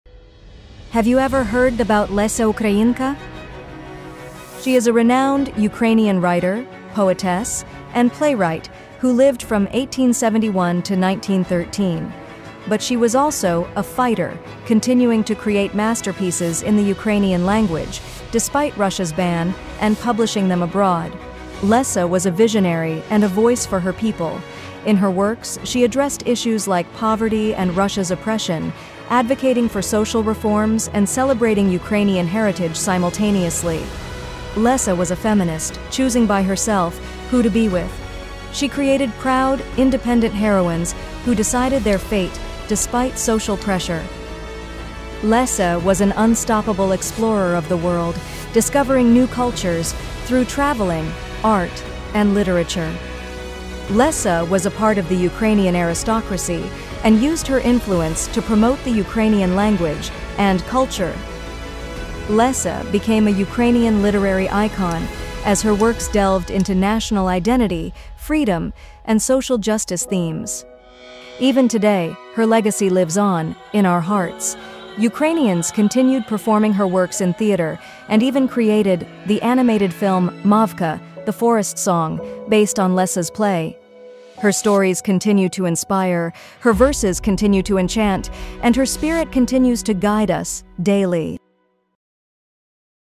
"Lesya Ukrainka: Ukraine’s Literary Legend"
🚨🌟 Exciting History Spotlight 🌟🚨 Meet Lesya Ukrainka, Ukraine’s literary giant and national heroine! Born in 1871 as Larysa Kosach-Kvitka, she was a poet, playwright, and activist. Despite battling tuberculosis and facing Russia’s ban on Ukrainian culture, she wrote iconic works like "The Forest Song" and "Contra spem spero!" Her progressive family, including her writer-activist mother Olena Pchilka, fueled her literary genius and patriotism. Lesya also championed social justice and women’s rights. Let’s celebrate her legacy and indomitable spirit! 🌿📚💪